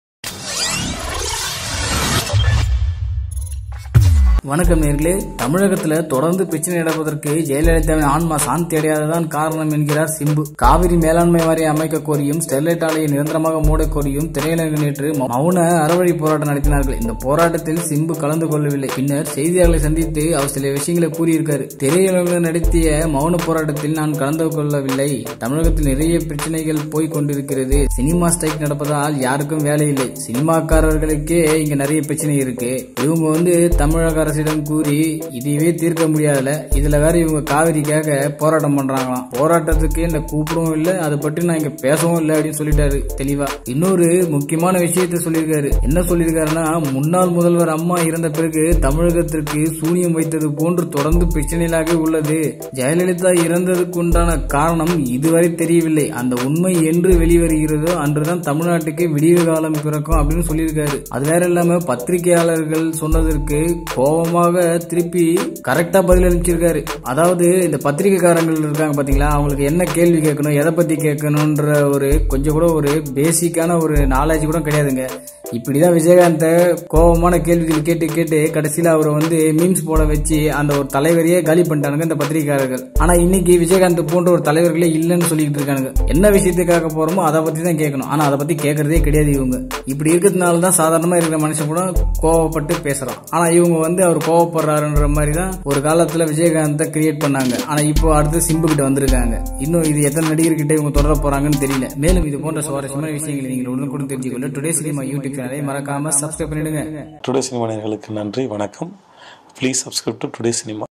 செய்தியாளர்களின் கேவலமான கேள்விகள், சிம்புவின் அதிரடி பதில்கள் - STR | Jayalalitha | Cauvery
ஜெயலலிதாவின் மரணத்திற்கு விடை கிடைத்தால் சிம்புவின் அதிரடி